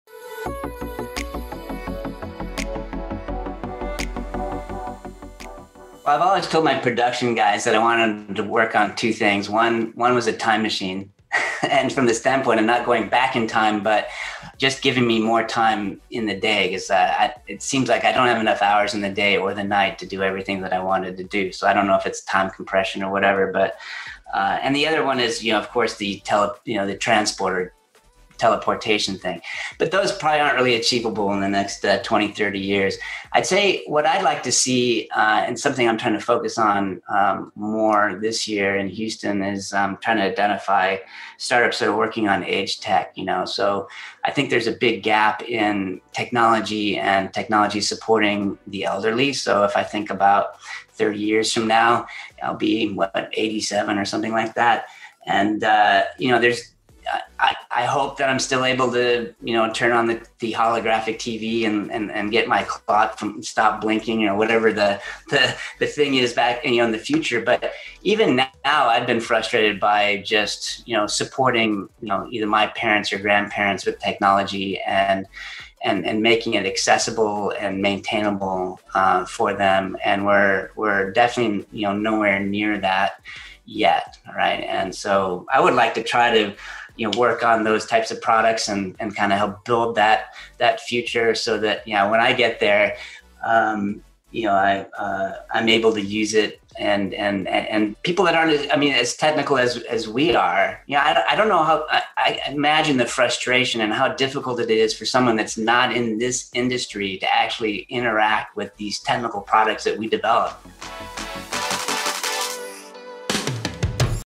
Chris Howard - Last Question
Speaker 1: (00:05)
Well, I've always told my production guys that I wanted to work on two things. One, one was a time machine and from the standpoint, I'm not going back in time, but just giving me more time in the day, cuz uh, I, it seems like I don't have enough hours in the day or the night to do everything that I wanted to do. So I don't know if it's time compression or whatever, but, uh, and the other one is, you know, of course the tele, you know, the transporter teleportation thing, but those probably aren't really achievable in the next, uh, 20, 30 years. I'd say what I'd like to see, uh, and something I'm trying to focus on. Um, more this year in Houston is, um, trying to identify startups that are working on age tech, you know, so I think there's a big gap in technology and technology supporting the elderly.

Speaker 1: (00:49)
So if I think about 30 years from now, I'll be what, 87 or something like that. And, uh, you know, there's, I, I hope that I'm still able to, you know, turn on the, the holographic TV and, and, and get my CLO stop blinking or whatever the, the, the thing is back, you know, in the future. But even now I've been frustrated by just, you know, supporting, you know, either my parents or grandparents with technology and, and, and making it accessible and maintainable, uh, for them. And we're, we're definitely, you know, nowhere near that yet. Right. And so I would like to try to, you know, work on those types of products and, and kind of help build that, that future so that, you know, when I get there, um, you know, I, uh, I'm able to use it and, and, and, and people that aren't, I mean, as technical as, as we are, you know, I, I don't know how I, I imagine the frustration and how difficult it is for someone that's not in this industry to actually interact with these technical products that we develop.